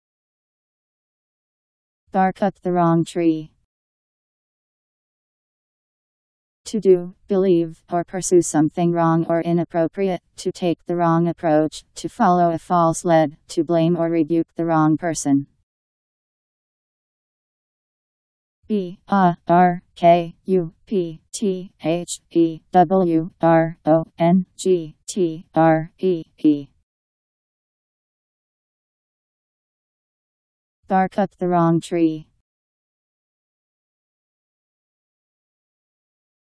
Bark up the wrong tree Meaning
Video shows what bark up the wrong tree means. To do, believe, or pursue something wrong or inappropriate; to take the wrong approach; to follow a false lead; to blame or rebuke the wrong person..  Bark up the wrong tree Meaning. How to pronounce, definition audio dictionary. How to say bark up the wrong tree. Powered by MaryTTS, Wiktionary